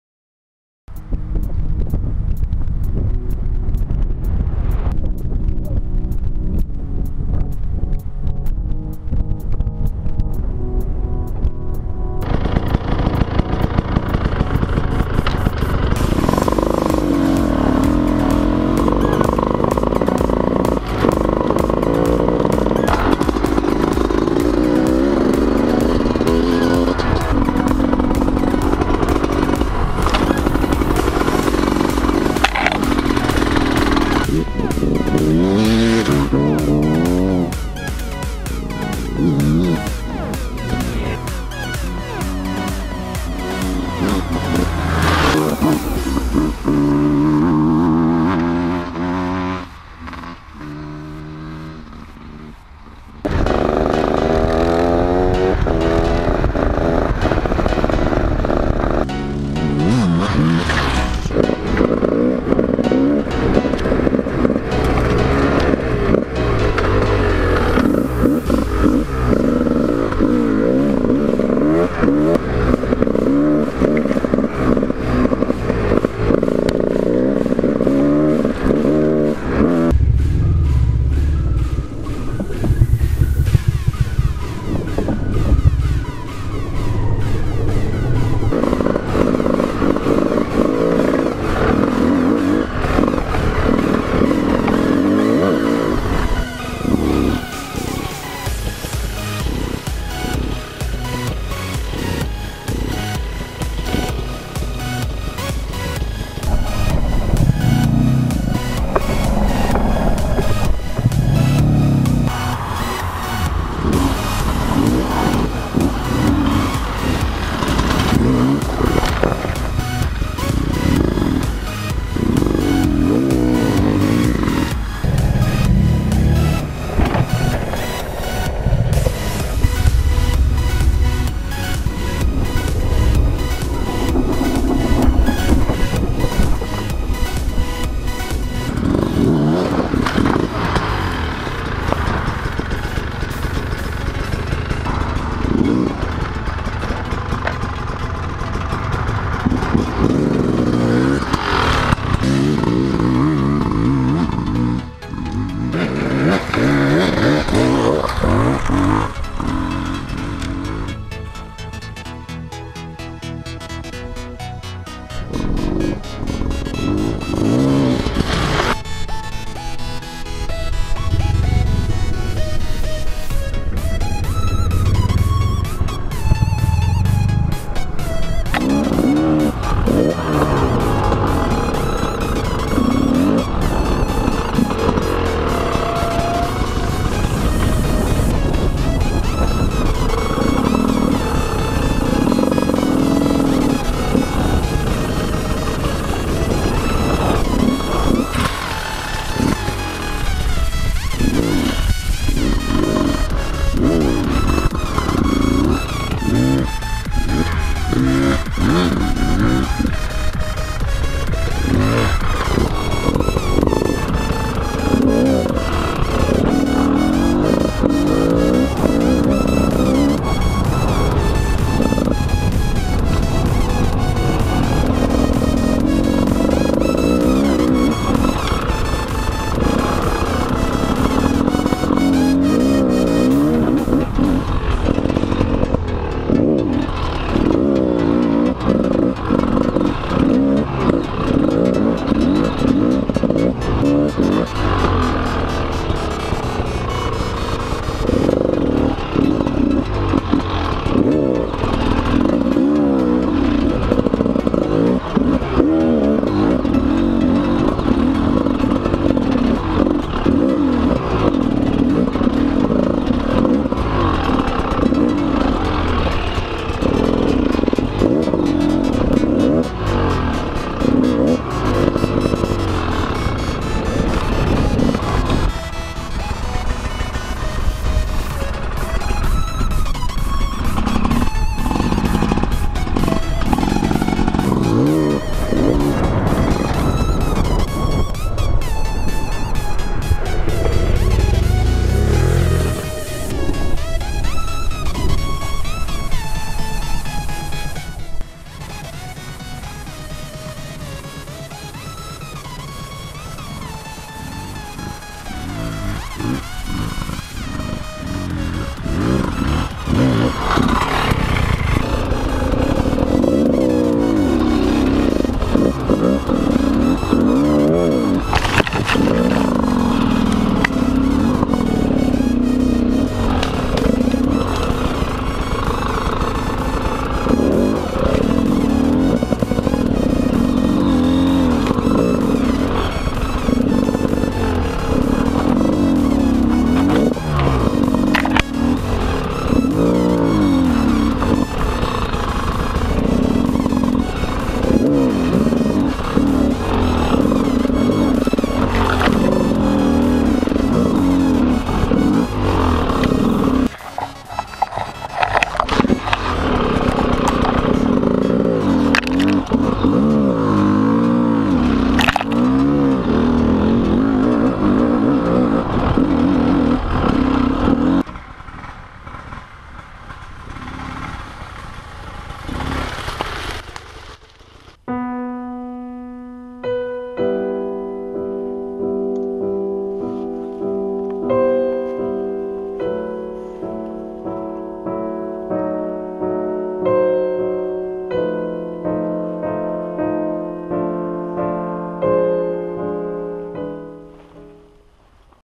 Fall Enduro | GasGas EC 300E Racing
Riding the 2stroke GasGas EC 300 E during the best riding season of the year. A winter video will follow soon, I'm just waiting for the snow right now ;)